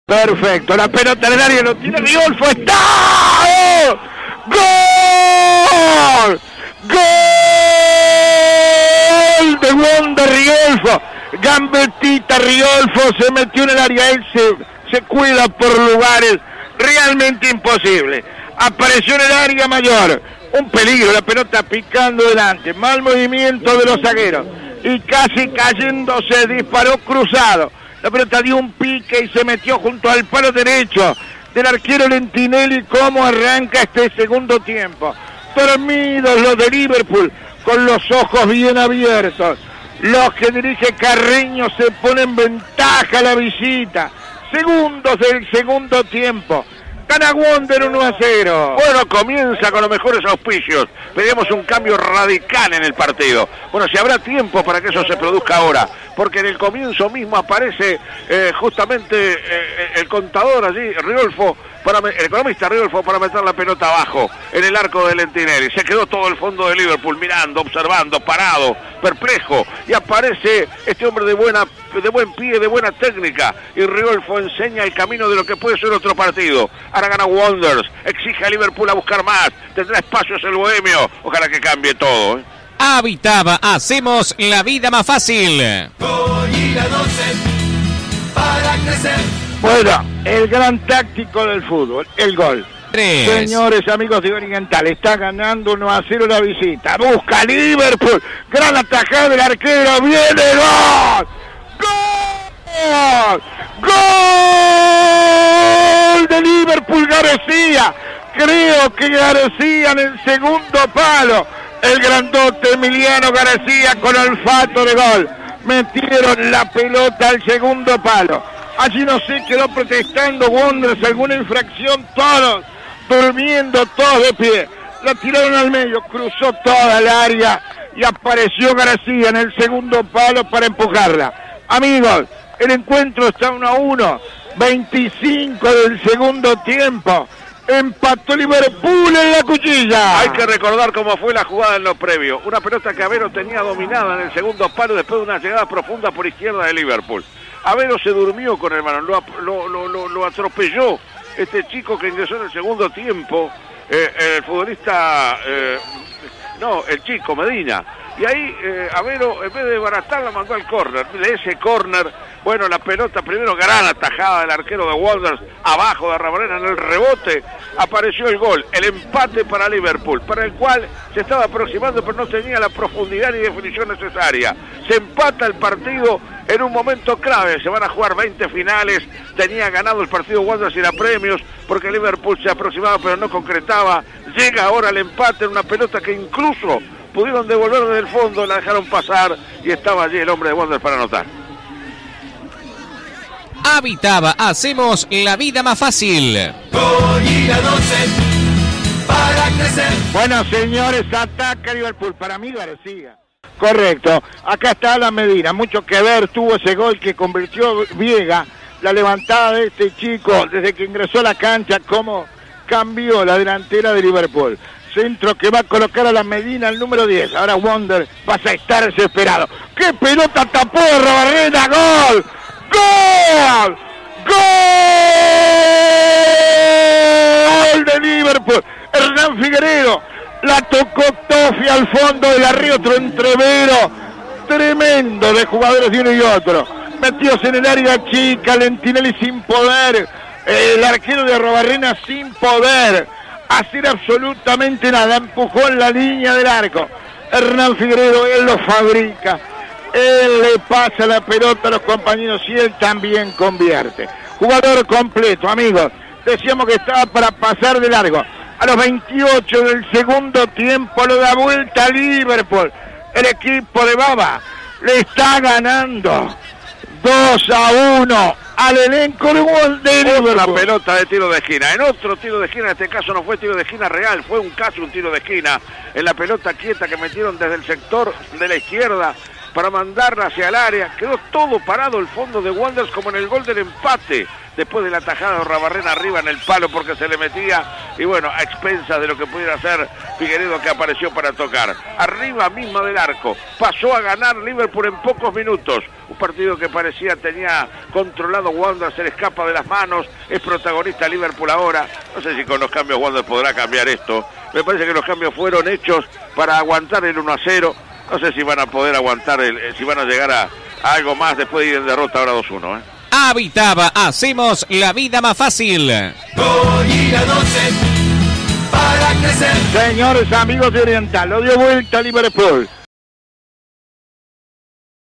Liverpool 2 Wanderers 1 - Apertura 2021 (por Fernando Martínez Casella)
Los goles del partido, por Fernando Martínez Casella y Nelson Foliatti para CX 12 Radio Oriental 770 AM.